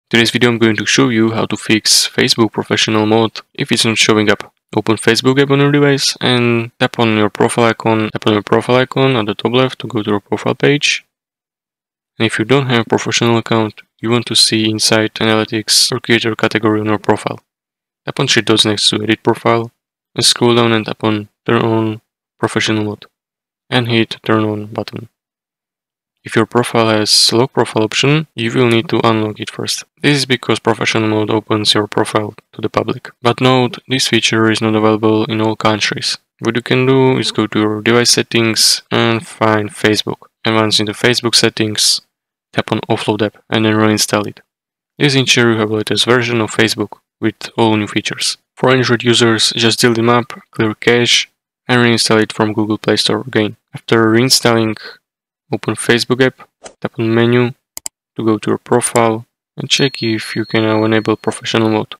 FIX Facebook Professional Mode not Showing (2024)
I'm going to show you how to fix Facebook Professional Mode if it's not showing up.

### Step-by-Step Guide

### Step 1: Open the Facebook App
1. **Launch Facebook:** Open the Facebook app on your device. These steps work for both iPhone and Android users.

### Step 2: Access Your Profile
1. **Tap Profile Icon:** Tap on menu icon at the right.
2. **Go to Profile:** Tap on your profile icon at the top left to go to your profile page.

### Step 3: Enable Professional Mode
1. **Check for Professional Mode:** If you don't have a professional account, you won't see insights, analytics, or a creator category on your profile.
2. **Tap the Three Dots:** Tap the three dots next to "Edit Profile."
3. **Turn on Professional Mode:** Scroll down and tap "Turn on professional mode." Hit the "Turn on" button.

### Step 4: Troubleshooting if Professional Mode Doesn't Show Up
1. **Check for Lock Profile:** If your profile has a lock profile option, you'll need to unlock it first. This is because professional mode opens your profile to the public.
   - **Note:** This feature is not available in all countries.

### Step 5: Reinstall Facebook
1. **Open Settings:** Go to your device settings.
2. **Select General:** Tap on "General" (for iPhone users).
3. **iPhone Storage:** Tap on "iPhone Storage."
4. **Find Facebook:** Scroll down and find the Facebook app.
5. **Offload and Reinstall:** Tap "Offload App" and then reinstall it. This ensures you have the latest version of Facebook with all new features.
   - **For Android Users:** Delete the app, clear the cache, and reinstall it from the Google Play Store.

### Step 6: Verify Professional Mode
1. **Open Facebook:** After reinstalling, open the Facebook app.
2. **Access Profile:** Tap on the menu, then go to your profile.
3. **Enable Professional Mode:** Check if you can now enable professional mode using the steps mentioned earlier.

By following these steps, you should be able to fix any issues with Facebook Professional Mode not showing up.